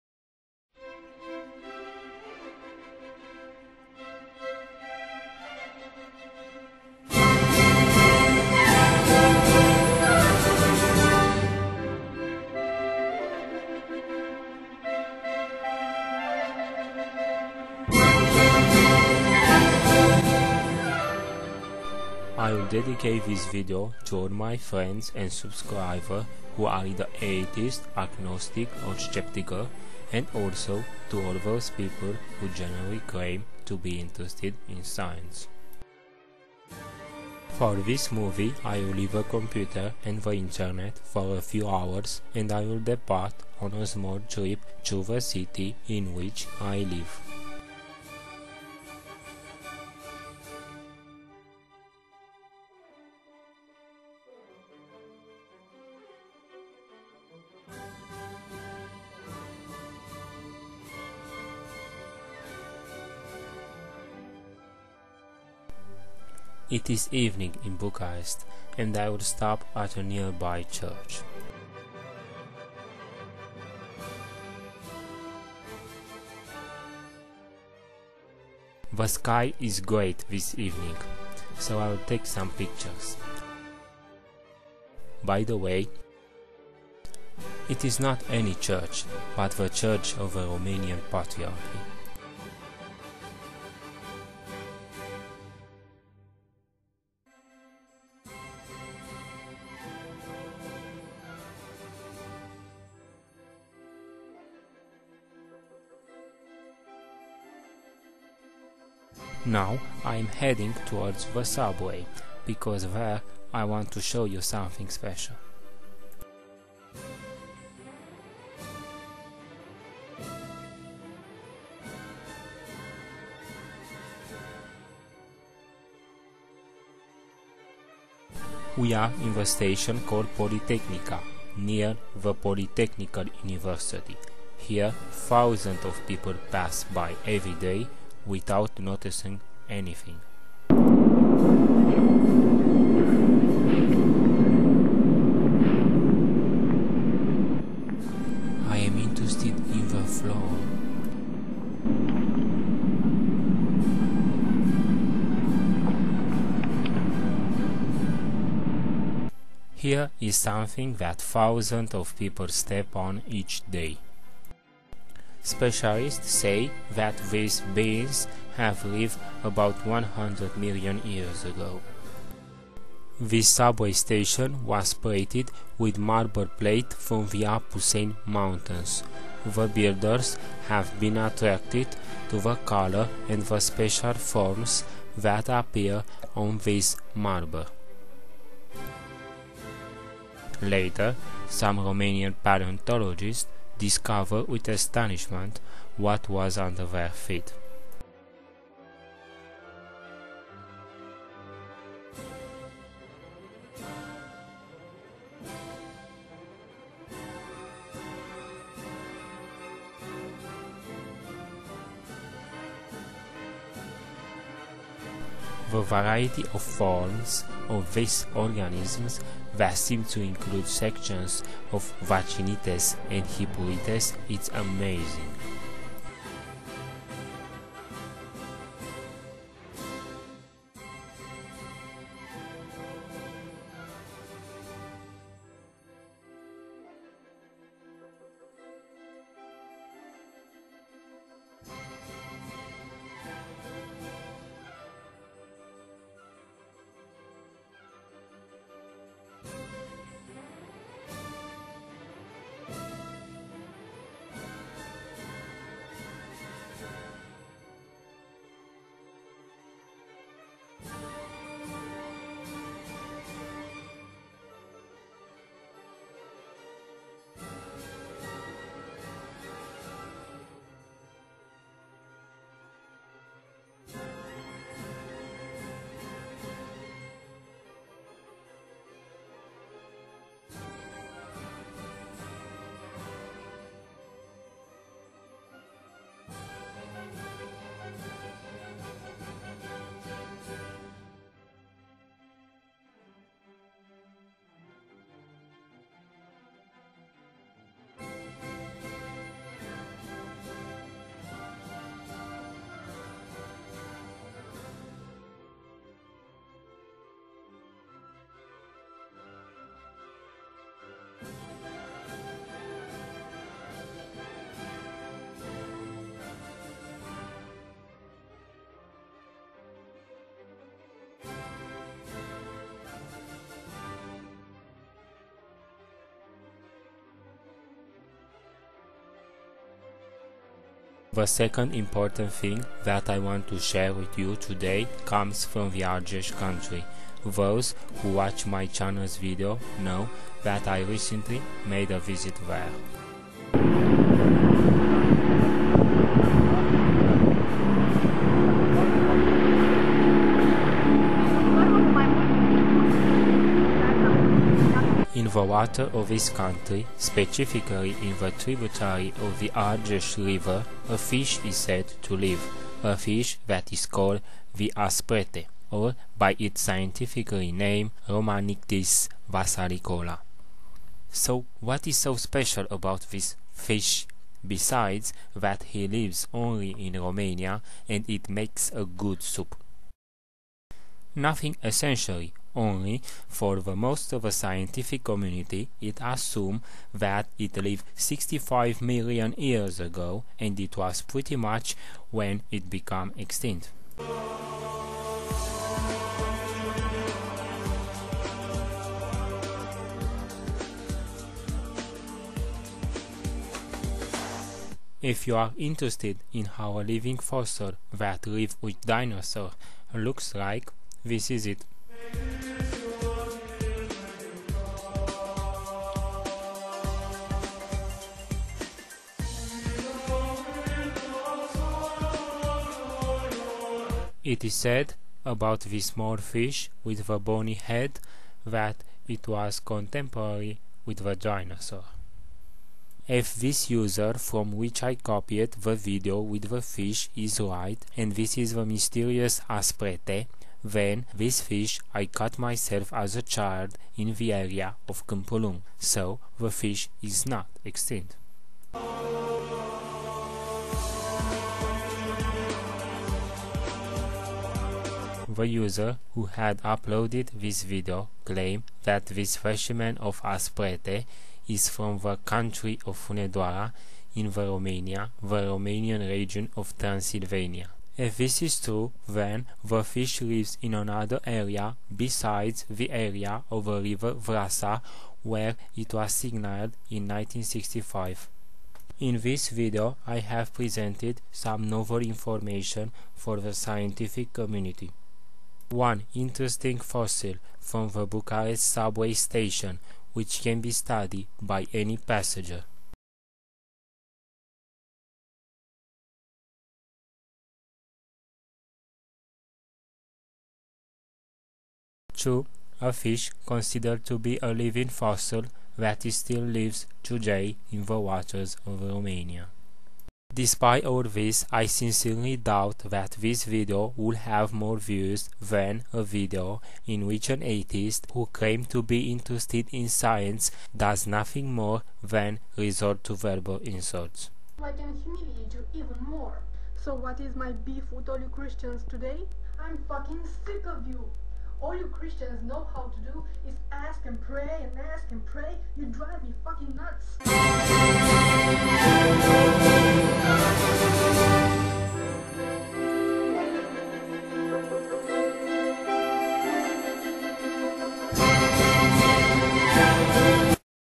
World Premiere - Never before seen fossils and a fish as old as the dinosaurs -
Most atheists have some concepts about life, similar to Nazis.
They believe that only they atheists, are true people humans, smart, beautiful, intelligent.

In this video I have presented some novel information for the scientific community.
1)Interesting fossils from a Bucharest subway station, which can be studied by any passer
sections of vaccinites? and  hippurites?

2)A fish considered to be a „living fossil, that still lives in the waters of Romania.
Aspretele or Romanichthys valsanicola.

Despite of all this, I sincerely doubt that this video will have more views than a video in which an atheist, who claims to be interested in science, does nothing more than resort to verbal insults.

Thus we have shown the true face of atheism.

References:
Romanichthys valsanicola

Hippurites Vaccinites.